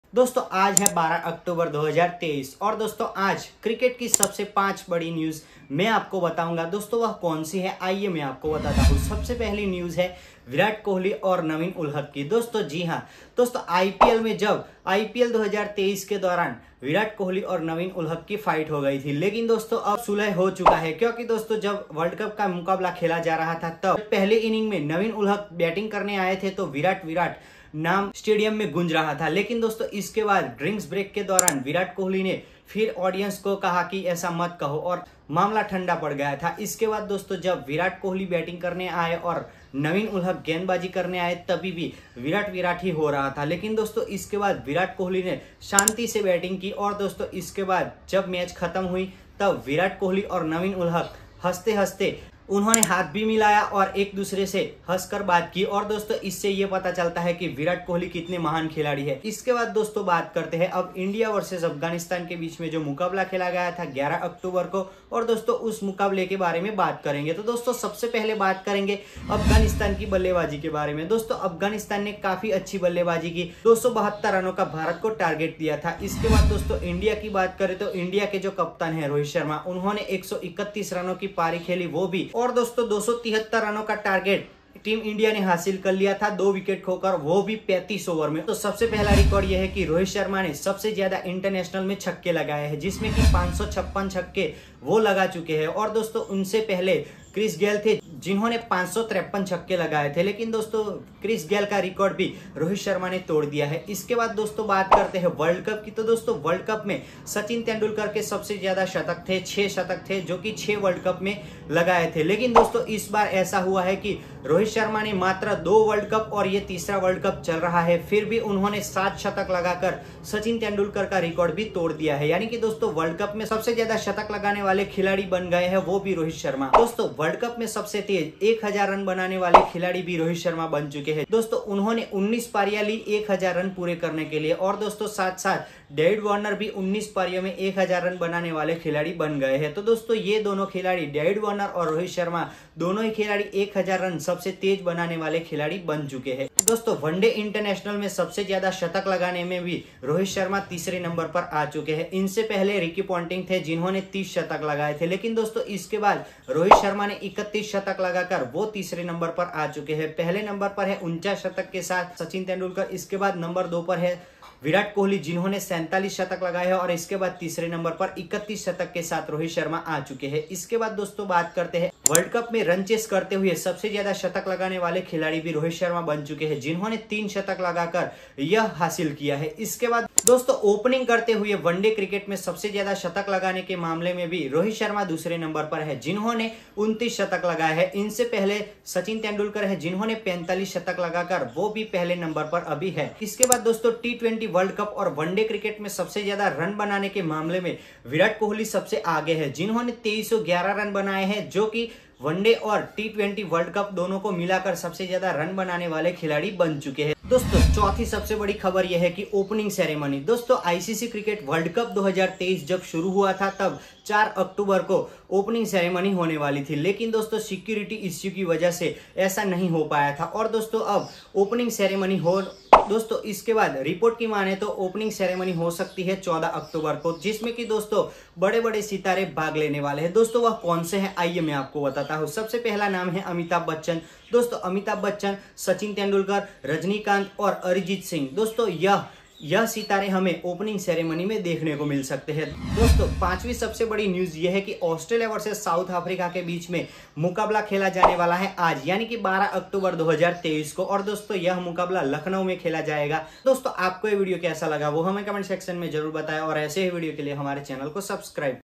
Virat And Naveen Just Friends 👬 | Rohit Sharma Rule in world Cup | Most Century in World Cup History
Cover Topic :
1.Virat Kohli And Naveen Ul Haq Just Friends
2.India vs Afghanistan Match Review
3.Rohit Sharma And Virat Kohli Record

    1.Most Century in World Cup History :
        Rohit Sharma 7 Century.

    2.Fastest 1000 Run In Wc :
        19 Inning Rohit Sharma.

    3.Most Century in ODI :
        Rohit Sharma 31 Century.

    4.Most Sixes in International :
        Rohit Sharma 556 Six.

     5.Most Century in ODI as Opner :
         Rohit Sharma 29 century as a opner

     6.Highest Run In ICC Tournament :
         Virat Kohli Hit 2311 run in icc Tournament

4.ICC Cricket World Cup 2023 Opening Ceremony
   14 October And Special guest
   Amitabh Bachchan, Superstar Rajnikant,Arijit Singh

5.Today Match 10 : Australia vs South Africa in Lucknow.